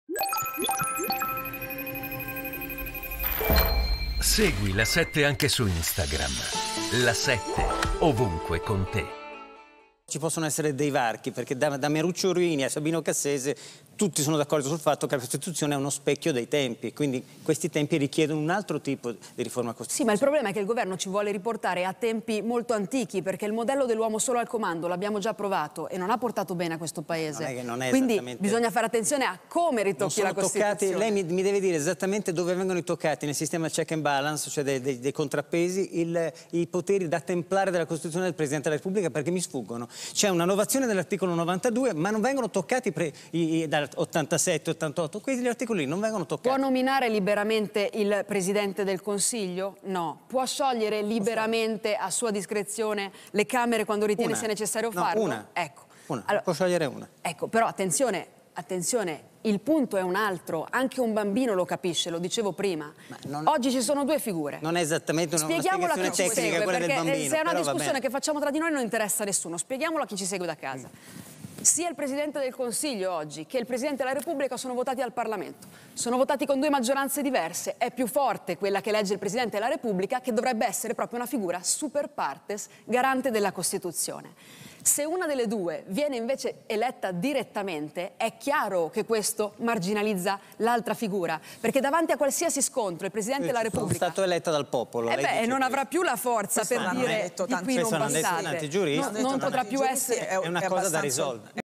Premierato, Elly Schlein: "Governo Meloni vuole uomo solo al comando. Lo abbiamo provato e non ...
Premierato, Elly Schlein: "Governo Meloni ci vuole riportare a tempi molto antichi, perché il modello dell'uomo solo al comando lo abbiamo già provato. E non ha portato bene".